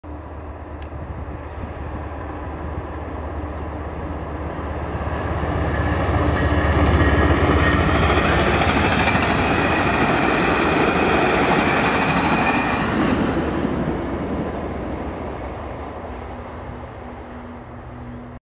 VIA rail passenger train #73, London ON, VIA 904 (16-Apr-2016)
2016 April 16, London, Ontario
VIA Rail train #73 westbound to Windsor
VIA #904 - P42DC (lead)
5 cars